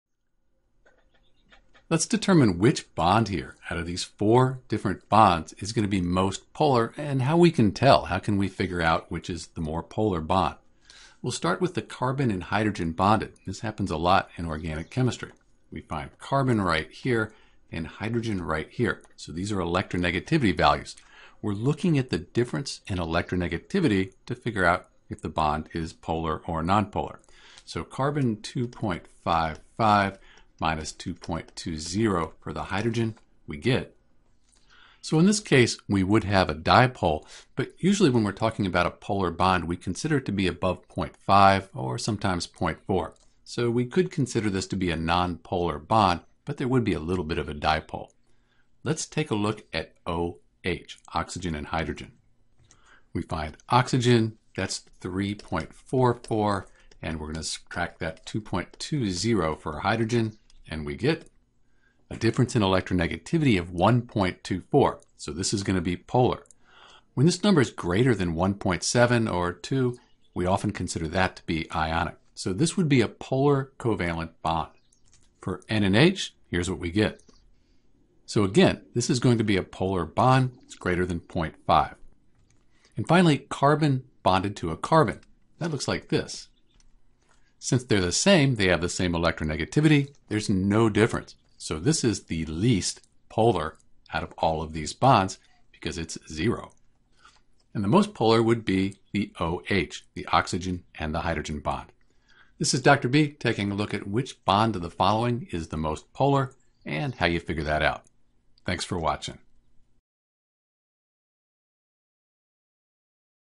Which Bond is the Most Polar: CH, OH, NH, or CC
A frequent chemistry quiz/exam question is to figure out what bond between atoms is the most polar.  In this video we determine whether CH, OH, NH, or CC has the most polar bond.

When determining the polarity of bonds it is important to know the trend on the Periodic Table.  Perhaps the easiest way to remember is that Fluorine (F) has the highest electronegativity value of all the elements.  Going away from F in any direction leads you to less electronegative elements.  Cesium (Cs) is often considered the least electronegative element.

The larger the difference in electronegativity between element, the more polar the bond.  We can use that information to determine which bond (CH, OH, NH, or CC) is more polar.